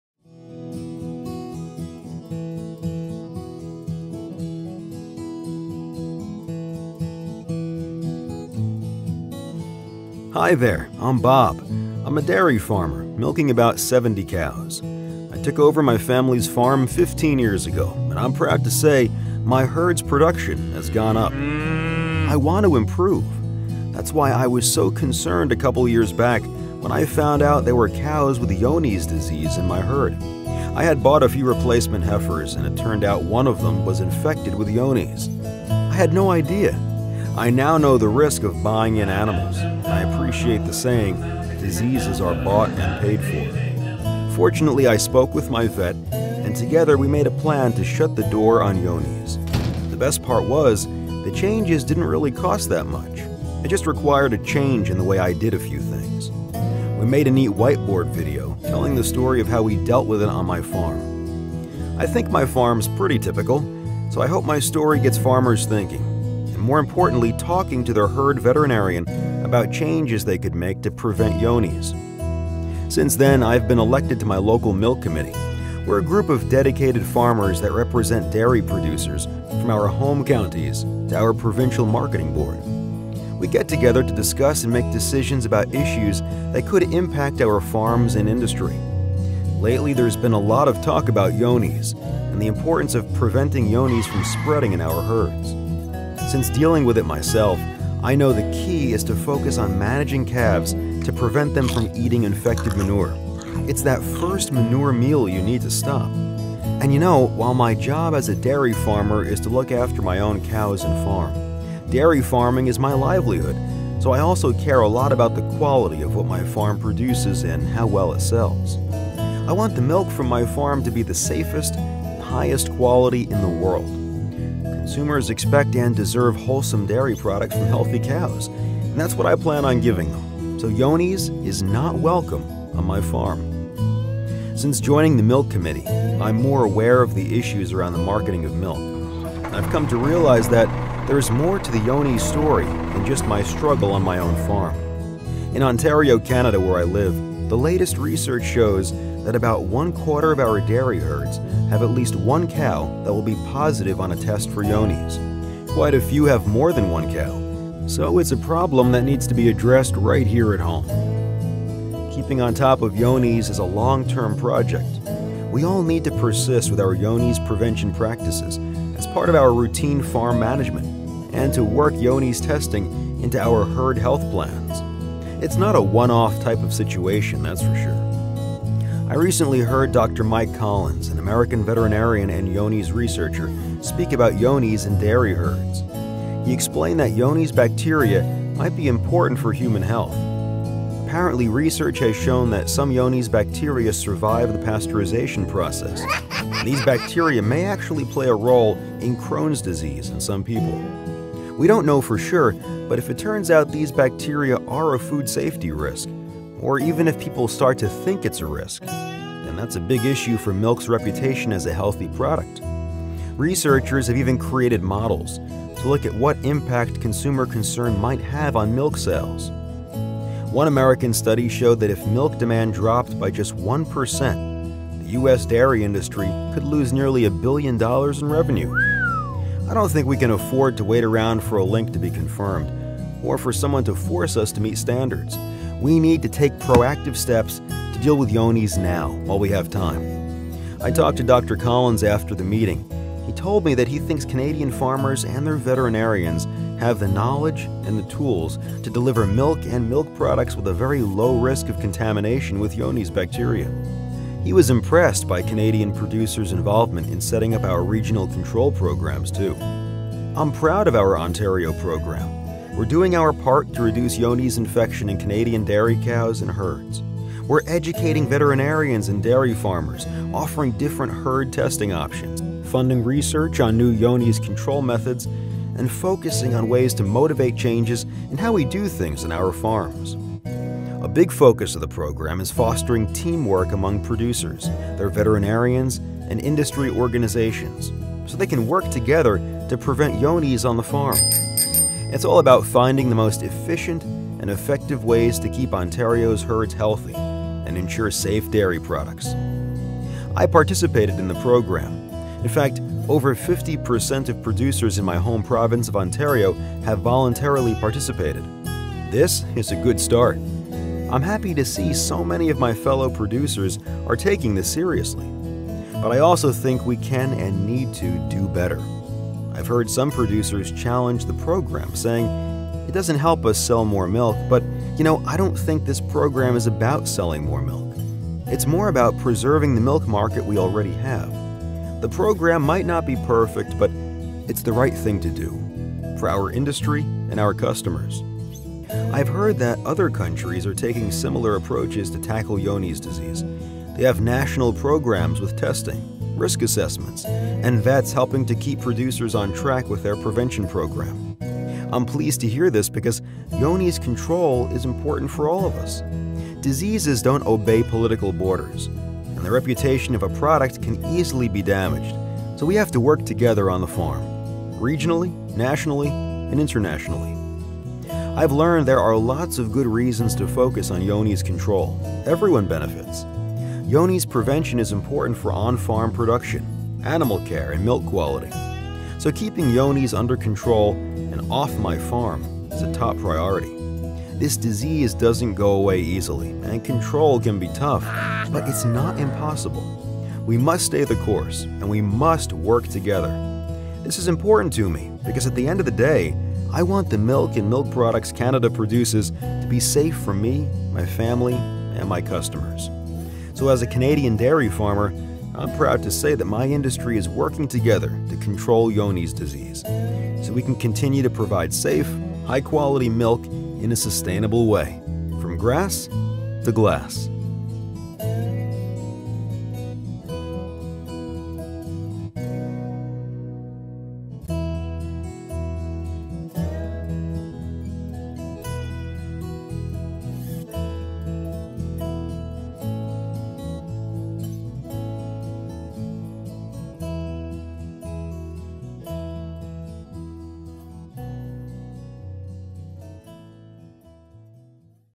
Johne's Disease in Canadian Dairy Herds: Impacts Across the Industry
Bob, a Canadian dairy farmer, tells his story of how he dealt with Johne's disease on his farm and worked with his veterinarian on changes they could make to prevent Johne's in the future. Johne's disease prevention and control is important for my farm and for the Canadian dairy industry as a whole.
Help do your part and learn more by watching this video.